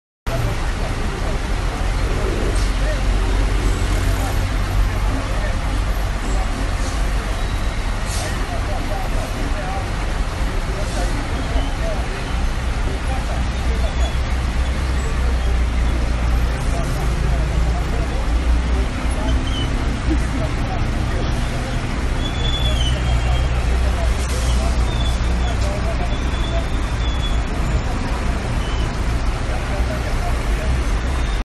Filman a agente de tránsito recibiendo coima
El agente de tránsito municipal de Ciudad del Este filmado al recibir dinero por parte de un automovilista fue identificado como Carlos Rubén Ocampos Figueredo. De acuerdo a un comunicado de la institución, "fue destituido sin más trámite"